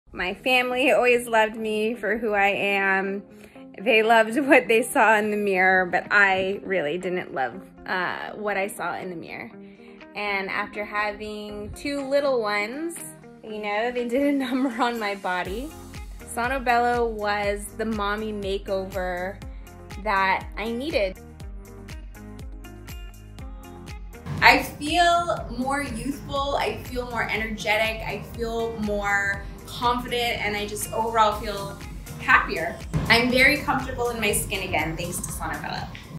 The Mommy Makeover Adrian Deserves
A busy mom of two boys, Adrian prioritized herself and got TriSculpt EX.  Now, she LOVES what she sees in the mirror.